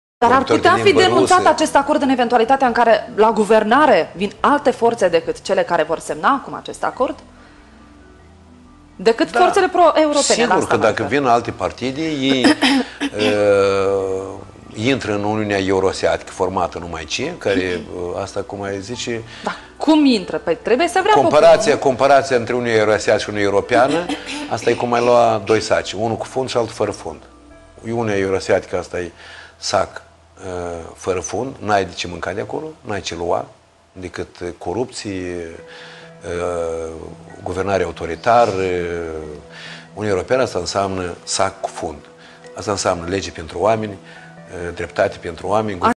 Гимпу сравнил Евразийский союз с мешком без дна
Председатель Либеральной партии сравнил Европейский и Евразийский союз с мешками, один из которых с дном, а другой -- без.
«Евразийский союз -- это мешок без дна. Там нечего искать, нечего брать, кроме коррупции, авторитарного правления. А Европейский союз -- это мешок с дном. Это означает законы и справедливость для людей»,  - сказал Гимпу.
Он не сомневается, что если к власти придут другие силы, они изменят внешнеполитическое движение Республики Молдова.